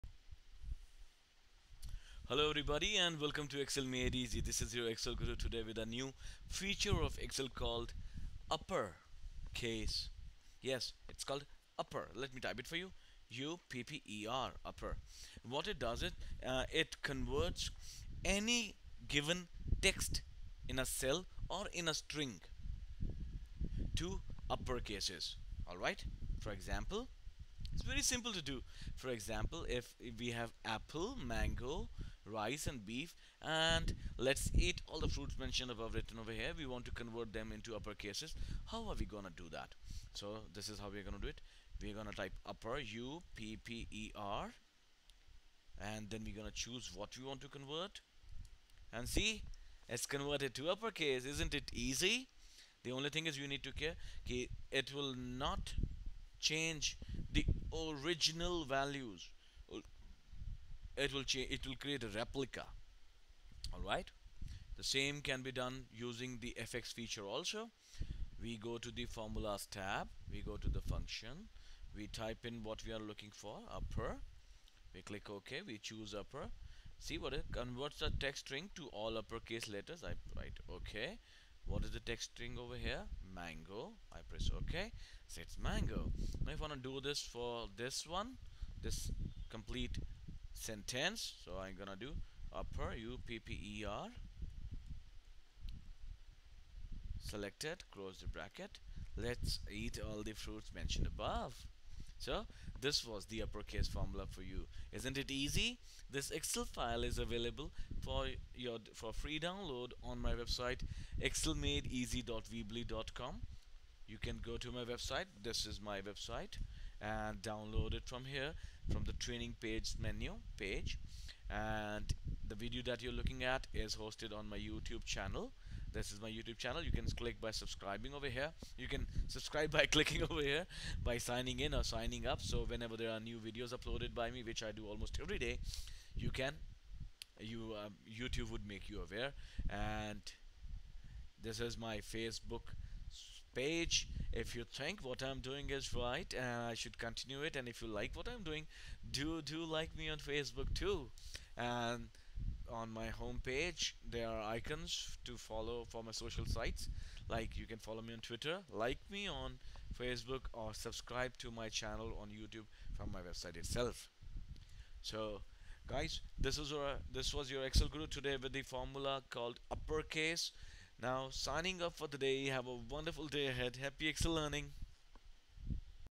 MS Excel Upper Case Formula
Upper case formula in excel can convert text to upper case with a single click! this particularity useful if you are working with text in Excel.

Whenever you want to work with text in excel then Upper case formula in Excel is the most used formula as it allows you to covert any written text to upper case in Excel.